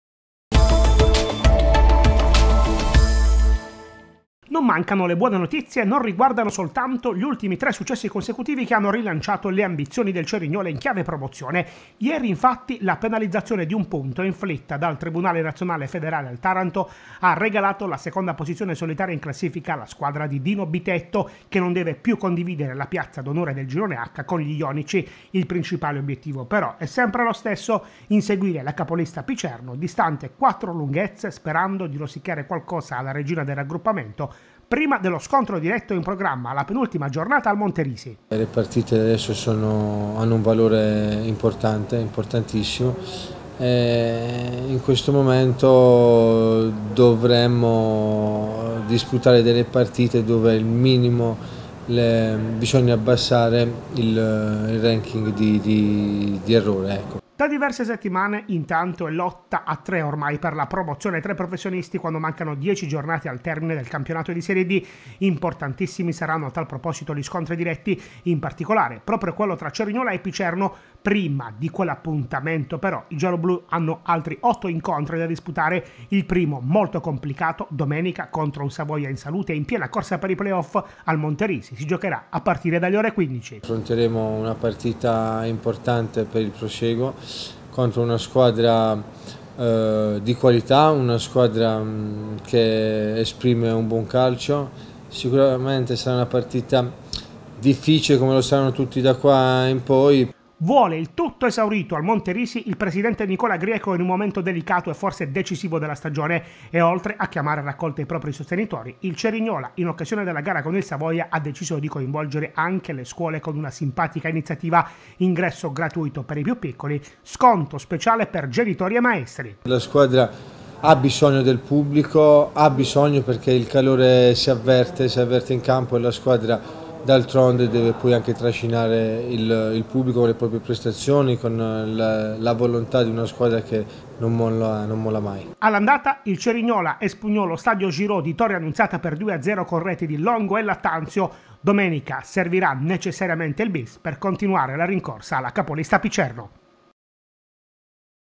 CERIGNOLA | La vetta sempre nel mirino: Cerignola ospita il Savoia
Per Giuseppe Abruzzese è una "partita insidiosa"